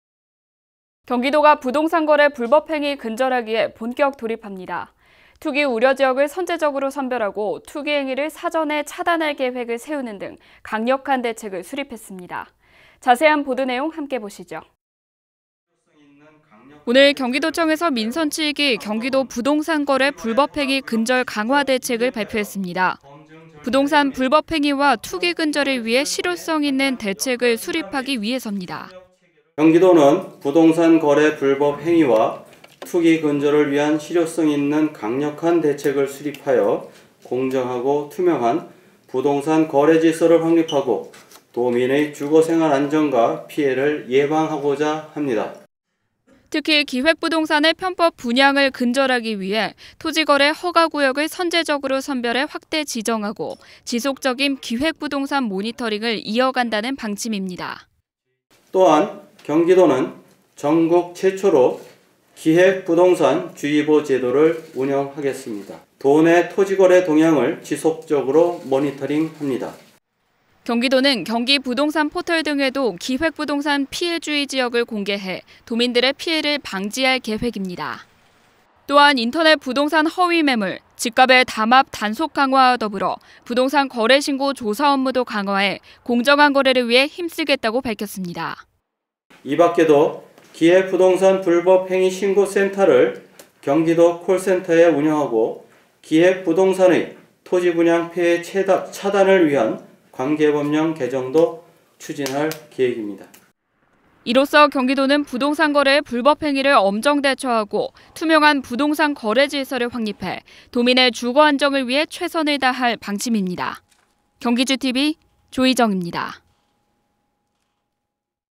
경기도, 부동산 거래 불법행위 근절 강화
[앵커멘트]
경기도가 부동산 거래 불법행위 근절하기에 본격 돌입합니다.
투기 우려 지역을 선제적으로 선별하고 투기 행위를 사전에 차단할 계획을 세우는 등 강력한 대책을 수립했습니다. 자세한 보도 내용, 함께 보시죠.

[리포트]
[자막]1. 경기도 ‘부동산 거래 불법행위 근절 강화 대책’ 정책브리핑/경기도청 5월 4일
오늘 경기도청에서 민선7기 경기도 ‘부동산 거래 불법행위 근절 강화 대책’을 발표했습니다. 부동산 불법 행위와 투기 근절을 위해 실효성 있는 대책을 수립하기 위해서입니다.

[자막]2. 김준태/도시주택실장
“경기도는 부동산 거래 불법 행위와 투기 근절을 위한 실효성 있는 강력한 대책을 수립하여 공정하고 투명한 부동산 거래 질서를 확립하고 도민의 주거 생활 안정과 피해를 예방하고자 합니다.”

[자막]3. 기획부동산 편법 분양 근절…토지거래 허가구역 선별 지정
특히 기획부동산의 편법 분양을 근절 하기 위해 토지거래 허가구역을 선제적으로 선별해 확대 지정하고 지속적인 기획부동산 모니터링을 이어간다는 방침입니다.

[자막]4. 김준태/도시주택실장
“또한 경기도는 전국 최초로 기획부동산 주의보 제도를 운영하겠습니다…도내 토지 거래 동향을 지속적으로 모니터링 합니다.”

[자막] 5. ‘경기부동산포털’에 기획부동산 주의지역 공개
경기도는 경기부동산포털 등에도 기획부동산 피해 주의 지역을 공개해 도민들의 피해를 방지할 계획입니다.

[자막] 6. 부동산 허위매물, 집값 담합 단속 등 부동산 거래신고 조사업무 강화
또한, 인터넷 부동산 허위매물, 집값의 담합 단속 강화와 더불어 부동산 거래신고 조사업무도 강화해 공정한 거래를 위해 힘쓰겠다고 밝혔습니다.

[자막]7.  김준태/도시주택실장
 “이밖에도 기획부동산 불법행위 신고센터를 경기도 콜센터에 운영하고 기획부동산의 토지 분양 피해 차단을 위한 관계 법령 개정도 추진할 계획입니다.”

[자막] 8. 투명한 부동산 거래 질서 확립해 도민의 주거 안정 이룰 것
이로써 경기도는 부동산 거래의 불법행위를 엄정 대처하고 투명한 부동산 거래 질서를 확립해 도민의 주거 안정을 위해 최선을 다할 방침입니다. 경기GTV 조이정입니다.

영상촬영 : 이효진, 나인선, 영상편집 : 김상환